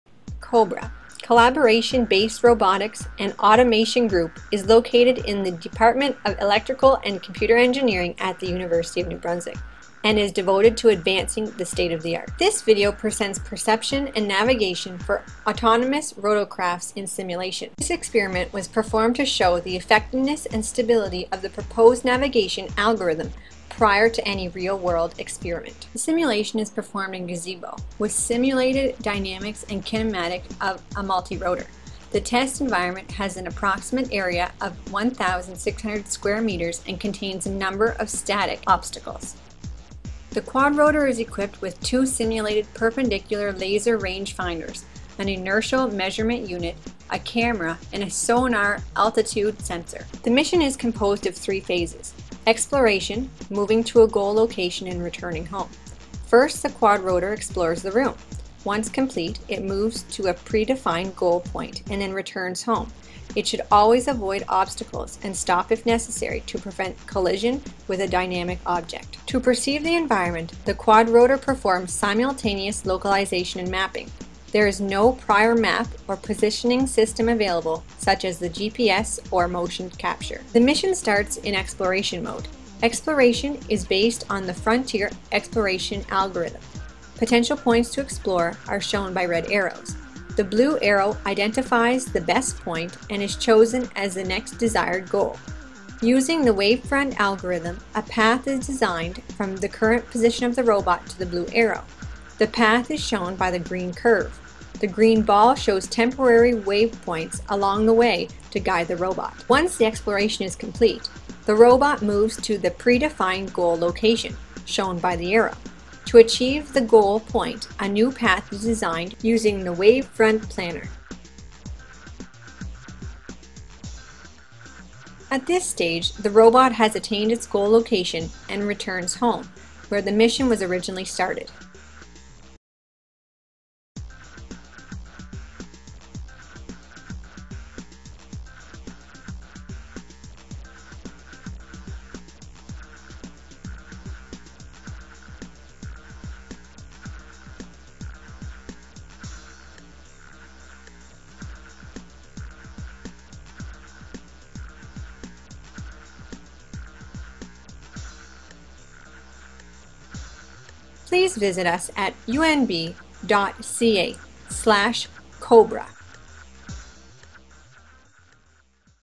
ROS/Gazebo Simulation
Perception and navigation for autonomous rotorcrafts: ROS/Gazebo simulation